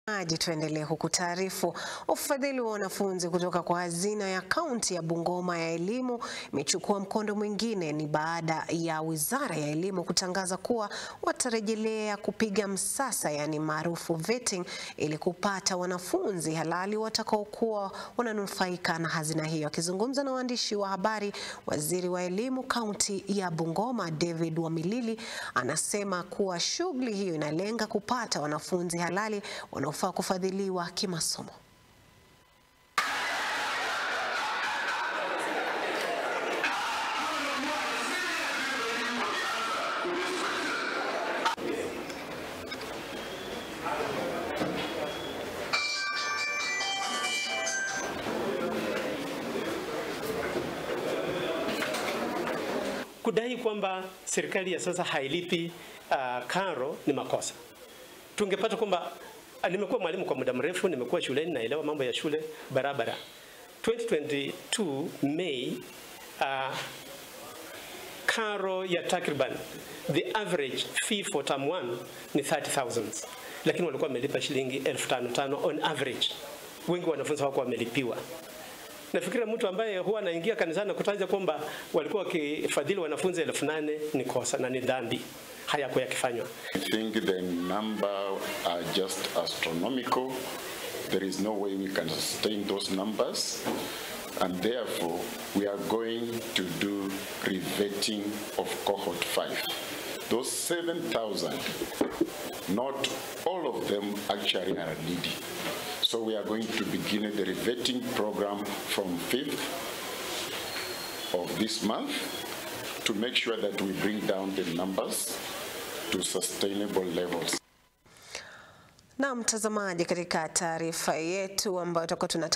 Ufadhili wa wanafunzi
Serikali ya Bungoma kurejelea msasa ufadhili kwa wanafunzi.

Wanafunzi waliopata zaidi ya 350 2022 kufadhiliwa.